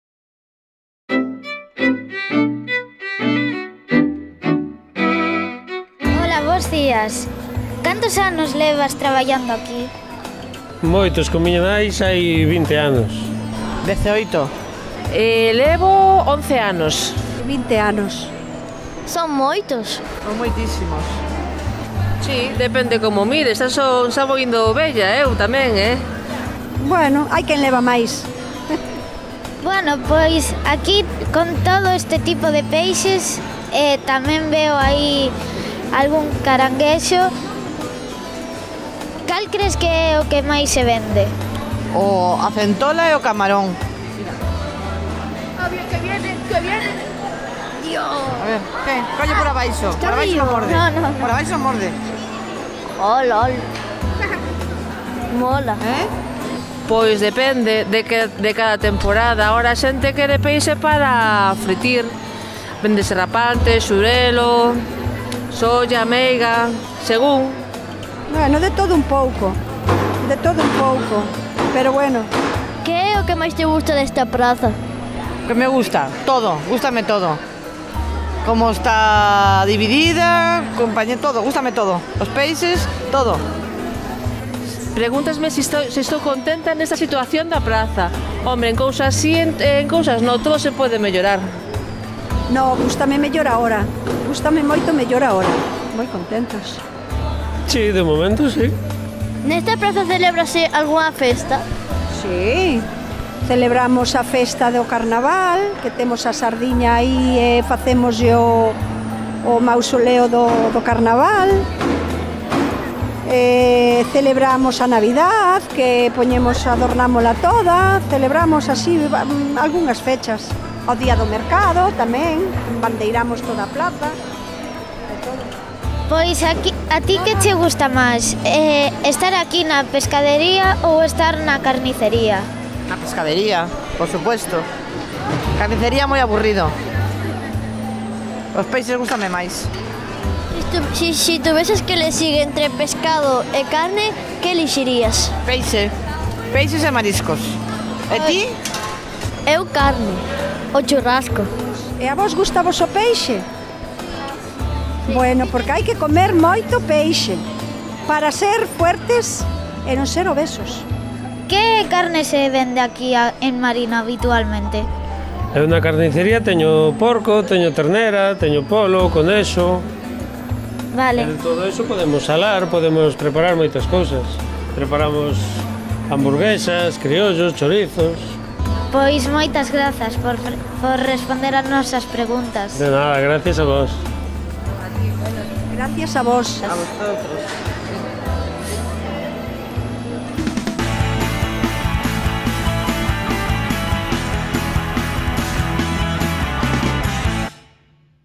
"Recunchos de Marín #3: Praza de Abastos"
-Traballo de investigación sobre as rúas e prazas de Marín, realizado polo alumnado de 6º de primaria.
-Rodado polo alumnado durante a andaina pola nosa vila o pasado venres 15 de febreiro de 2019.
-Temática: Praza de Abastos.
-PDI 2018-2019: "Marín, de todo un chisquín".
-Entrevistados/as: Begoña, Esther, Paca e Jacobo (traballadores/as da praza).
-Equipo de Dinamización da Lingua Galega, titorías e Obradoiro de Audiovisuais (edición).
-Mestres e mestras coordinadores: Charo Martínez Epifanio, Milagros Paz García, María Jesús Aldao Pampín, Yeray González López e Víctor Alfonso Castro Martínez.
-O recuncho de Tambo.
-C.E.I.P. Do Carballal de Marín.